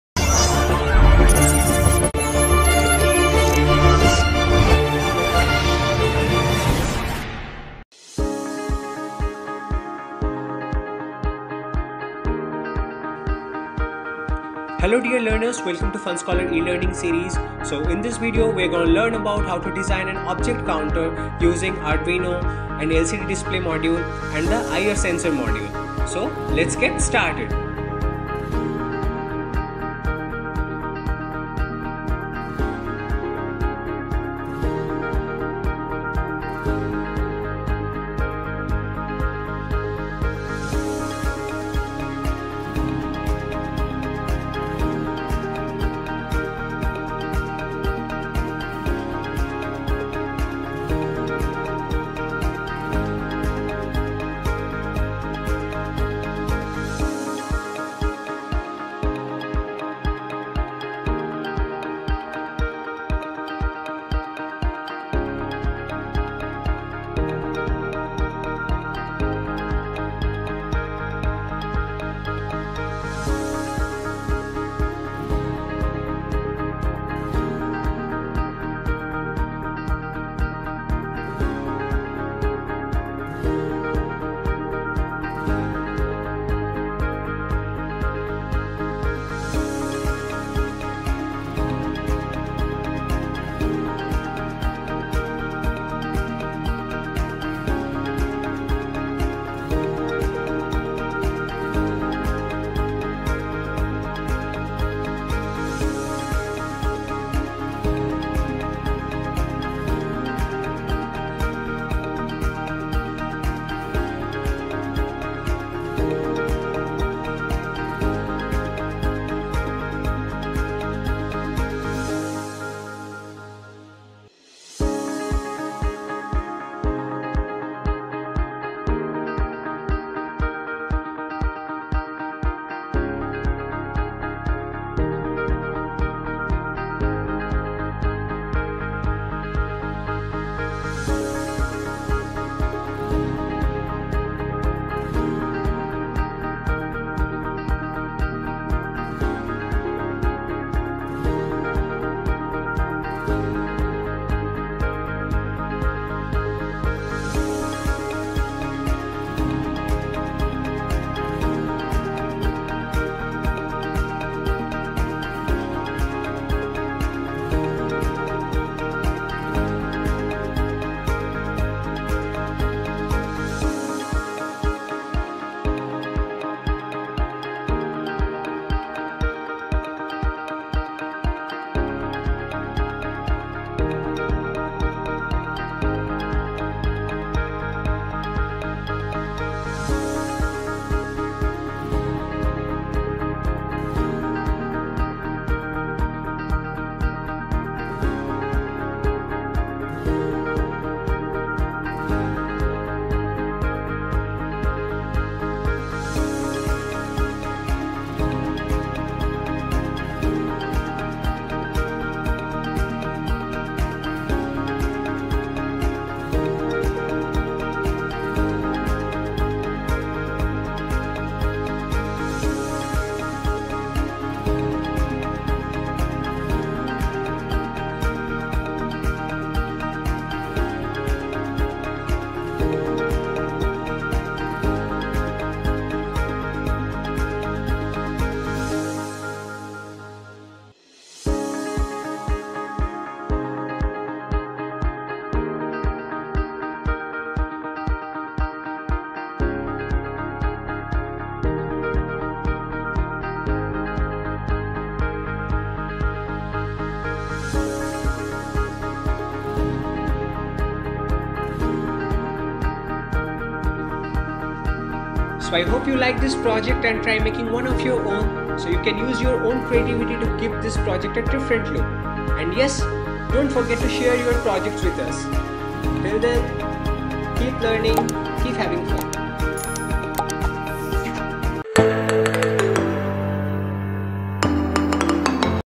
Object Counter Using IR Sensor Project # 13
An object counter is used to count huge number of elements passing through a specified space. These are used in places where counting elements manually is not possible. It functions by counting the number of times an element is passing through the sensor.

Timestamp:
0:00 - Intro Video
0:08 - Preview
0:18 - Learning Objective of this Video
0:28 - Construction/Building of Object Counter
3:56 - Arduino Code Burning
4:18 - Working Application